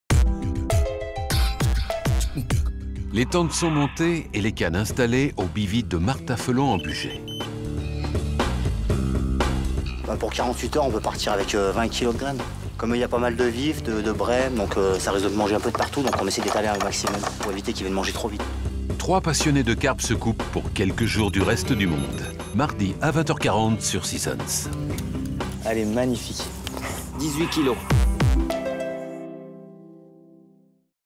Le Bivy
En janvier 2010, Seasons vous emmêne passer 48 heures sur un carpodrome avec trois carpistes passionnés. Bonnes carpes !